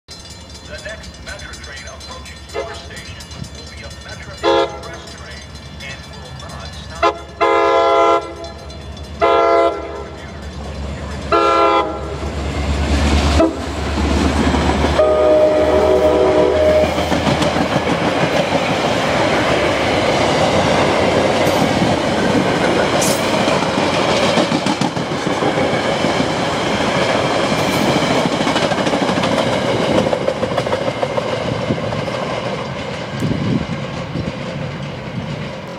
Metra MP38PH #410 Coasts Through Blue Island Station
While my friend, who isn't into trains as much, went to Chicago on vacation, I asked him to film a couple of trains for me. He's one at Metra's Blue Island Station. Metra MP38PH-3S #410 coasts through Blue Island. Enjoy! (Taken 8/11/11 at 5:34 PM)